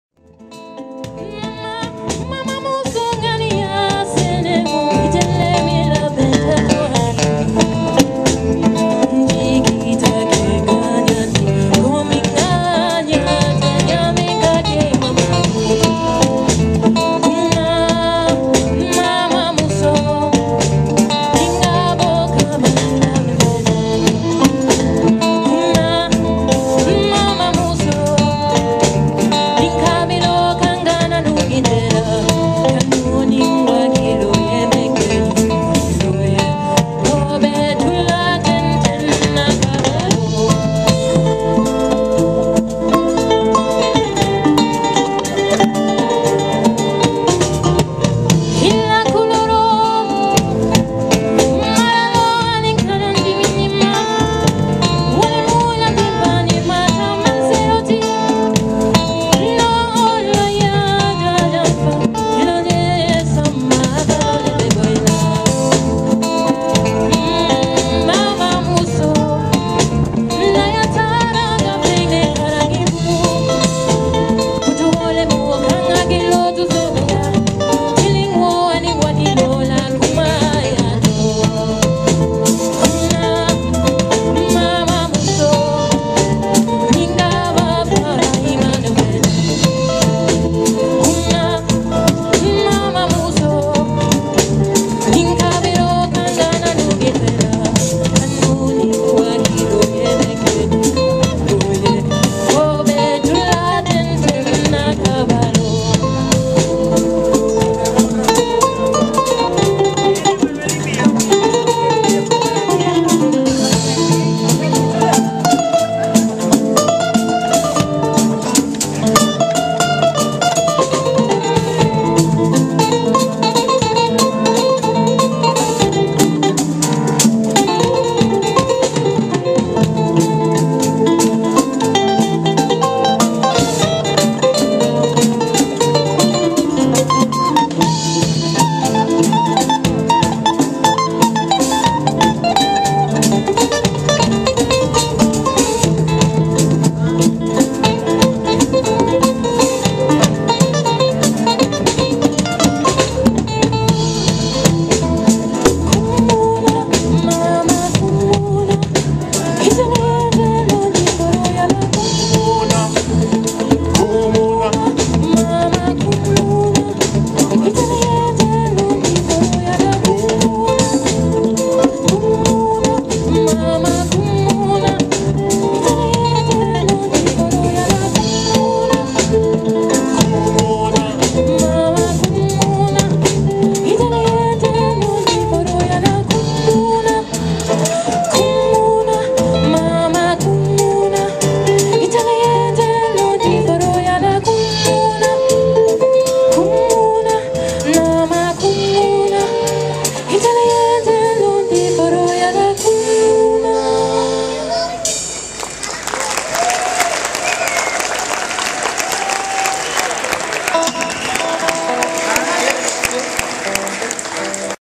Sona Jobarteh
Sona Jobarteh plays outside the National Theatre, London, as part of the Watch This Space festival